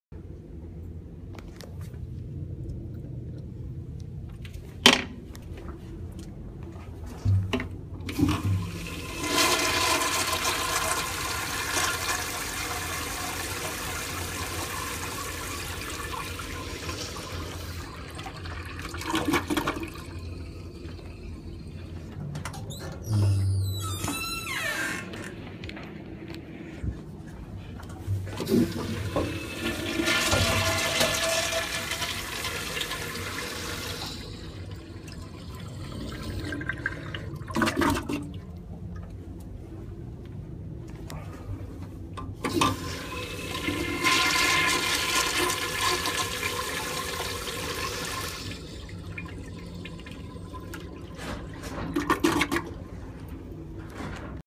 671. Two "Standard" Afwall Toilets.
These toilets are SUPER AMAZING! These toilets have AMAZING FLUSHES! This video was filmed in the same building as the last video.